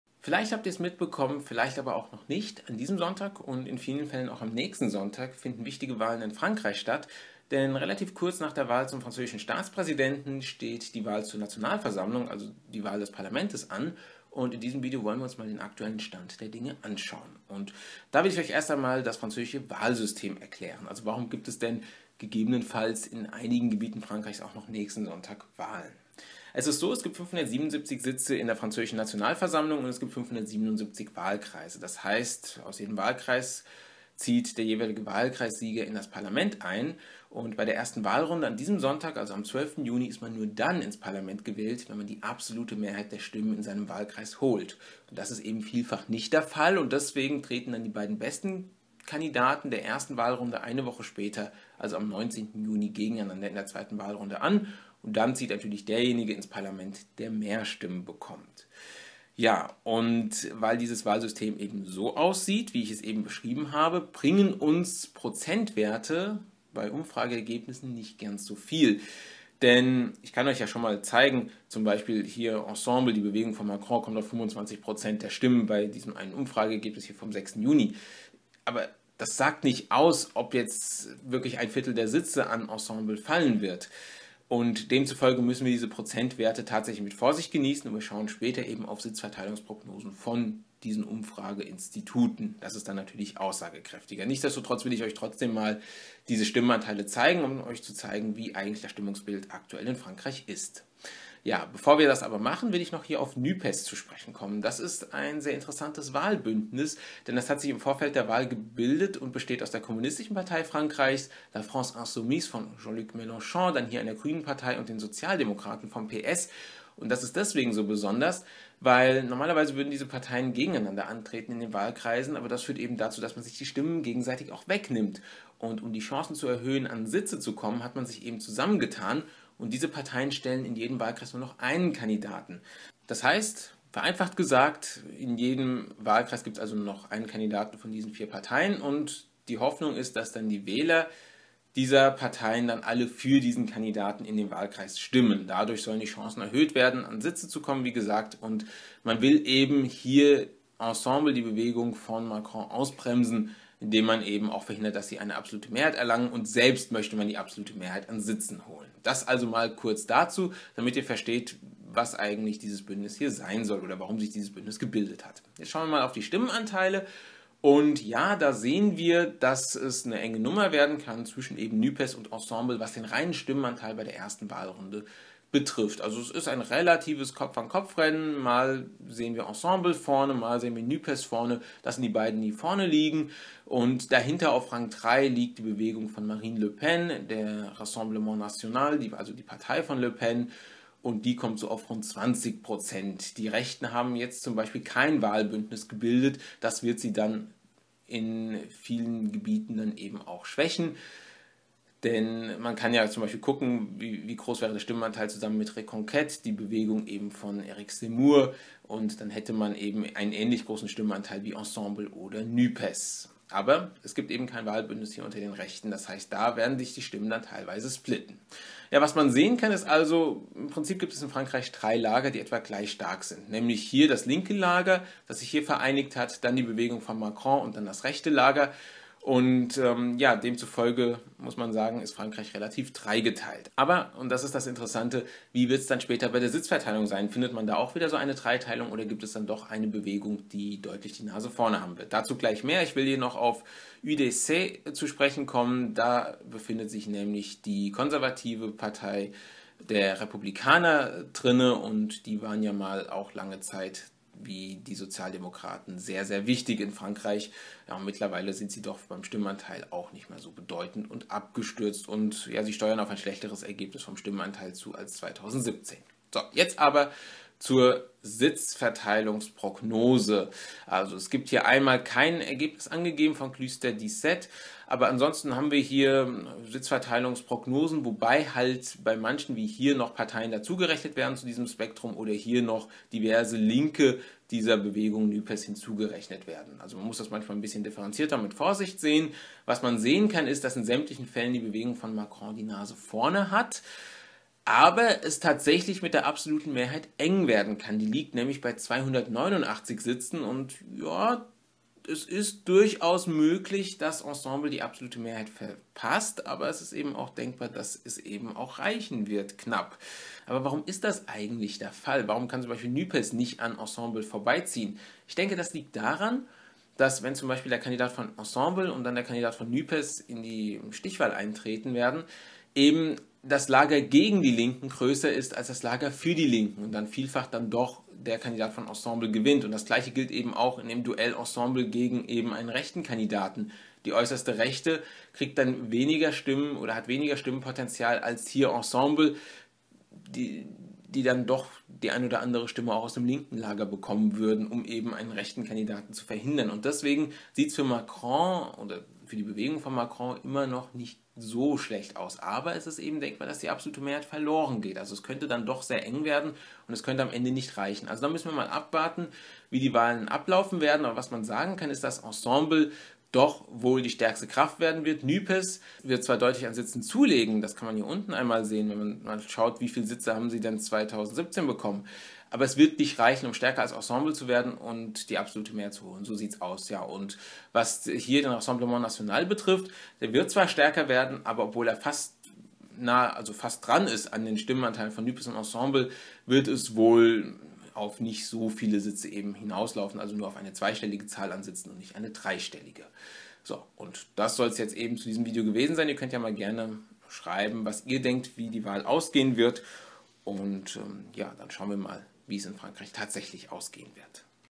Wahl der Nationalversammlung in Frankreich (12.+19.06.2022) - Wahlsystem und aktuelle Umfragen
Diesen und in den meisten Fällen nächsten Sonntag findet die Wahl zur Nationalversammlung in Frankreich statt. Wie das Wahlsystem aussieht und wie der aktuelle Stand der Dinge lautet, schauen wir uns in diesem Video an.